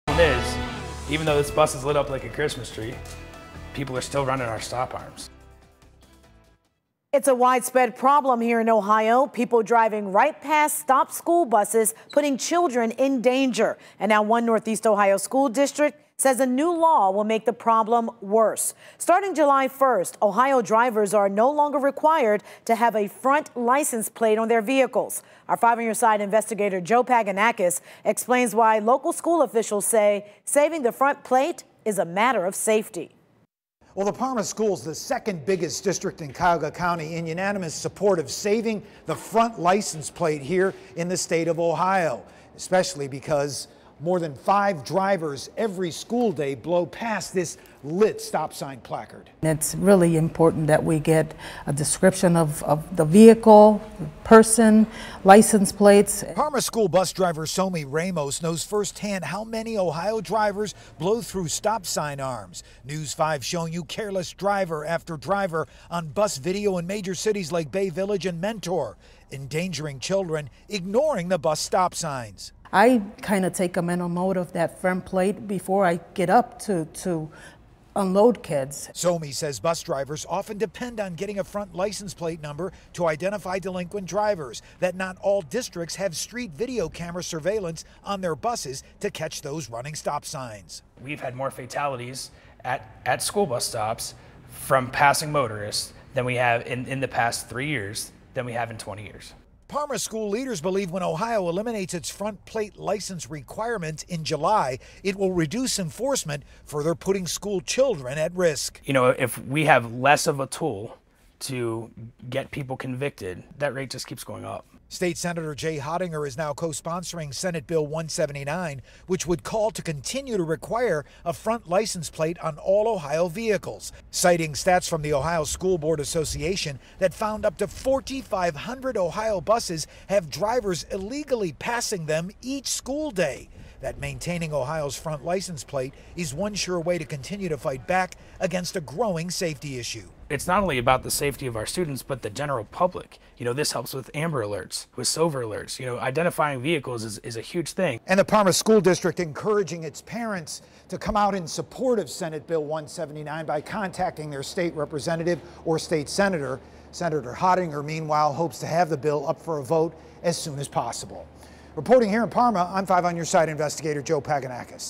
Local school bus drivers join fight to save Ohio front license plates
The Parma School District and its bus drivers are now joining the fight to maintain the need for a front license plate on Ohio vehicles.